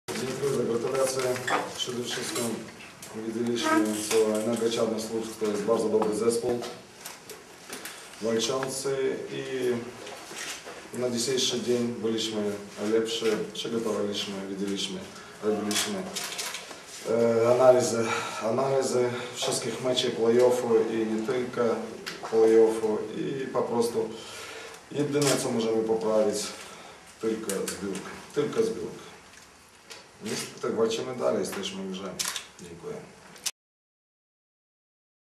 Konferencja po meczu: Igor Griszczuk, trener Anwilu Włocławek
Anwil Włocławek - Energa Czarni Słupsk 104:75